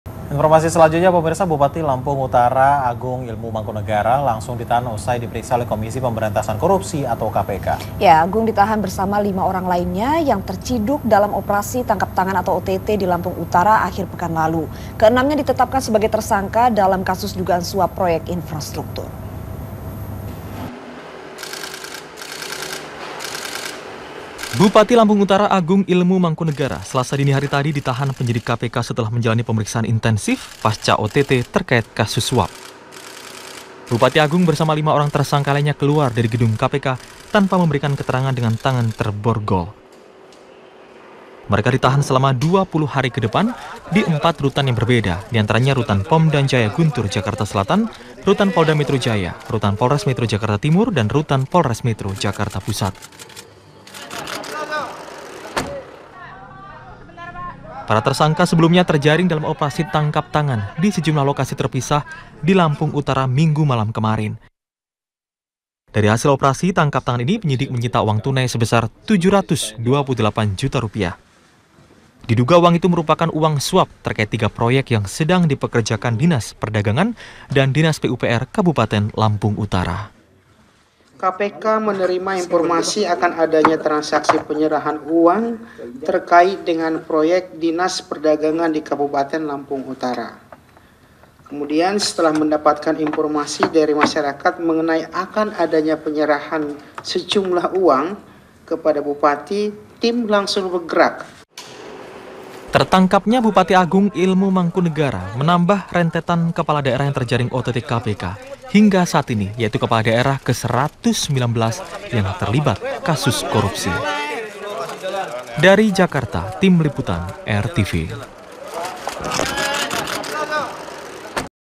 Bupati Lampung Utara Ditahan KPK
Agung Ilmu Mangkunegara, Bupati Lampung Utara, ditahan setelah menjalani pemeriksaan intensif setelah terjerat OTT terkait kasus suap, minggu malam (6/10/2019) kemarin. Penyidik menyita uang sebesar 728 juta rupiah, hasil uang suap dari 3 proyek yang sedang dikerjakan oleh Dinas Perdagangan dan Dinas PUPR Kabupaten Lampung Utara.